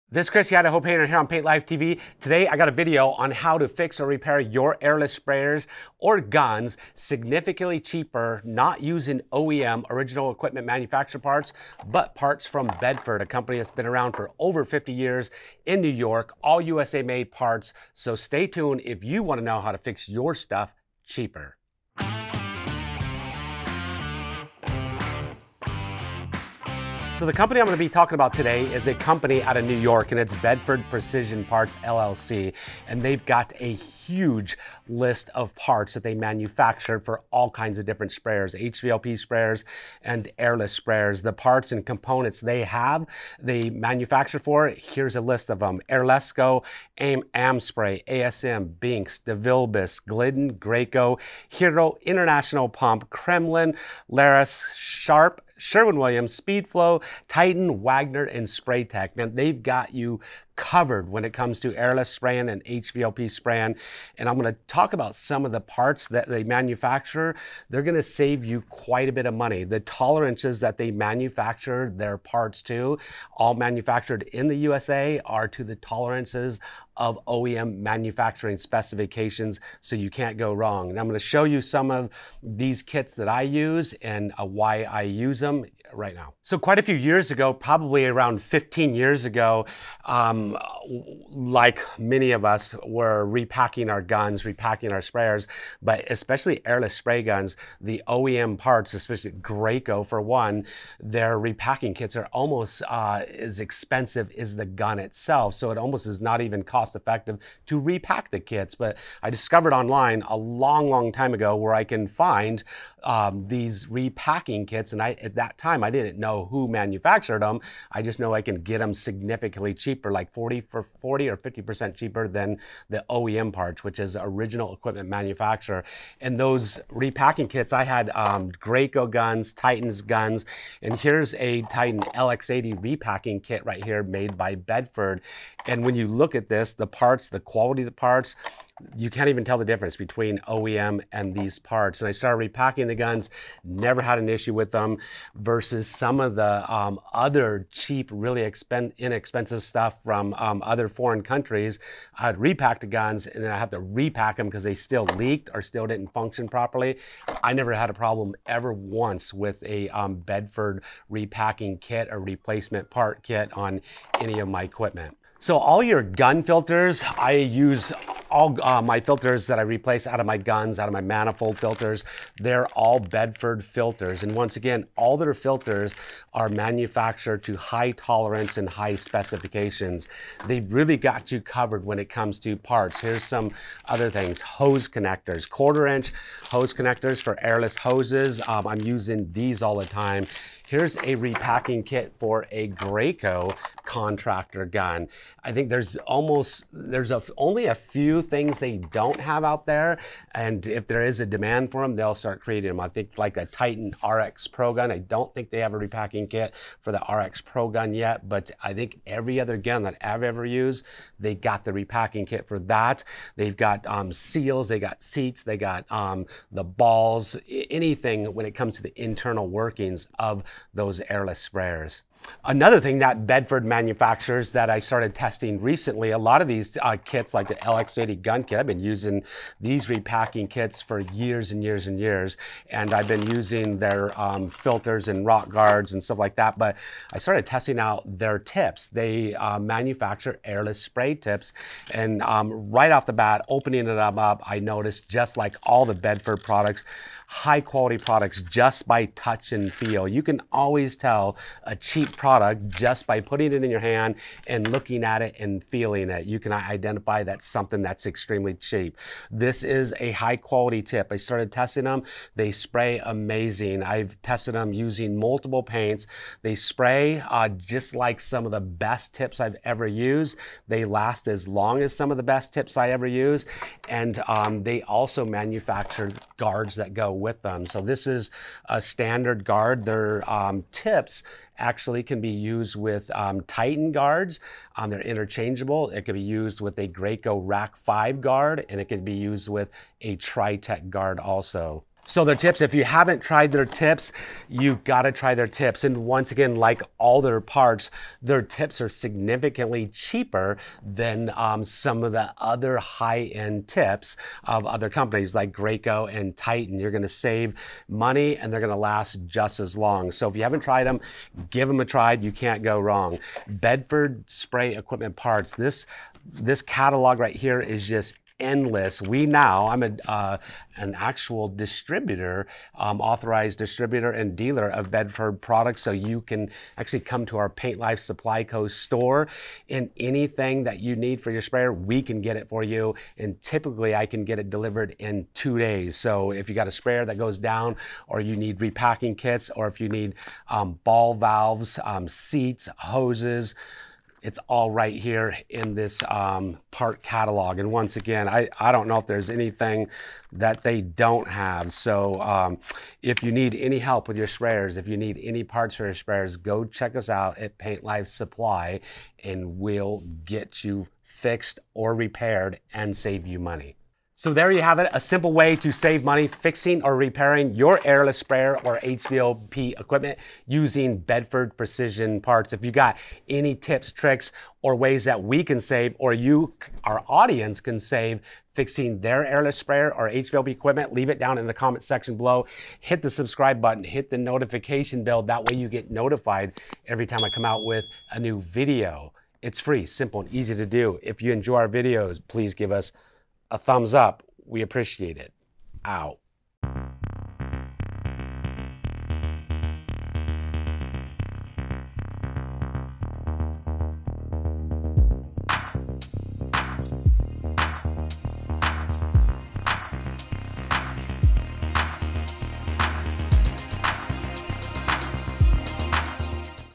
Quick and Cheap Airless Sprayer Repair! How to Save Money Repairing Your Own Airless Sprayers.
Quick and cheap airless sprayer repair done using Bedford products to fix all major  brands of airless paint sprayers like Titan, Graco,  Wagner, and Devilbliss with the Idaho Painter on Paint Life Tv.

2 Corinthians 1:21-22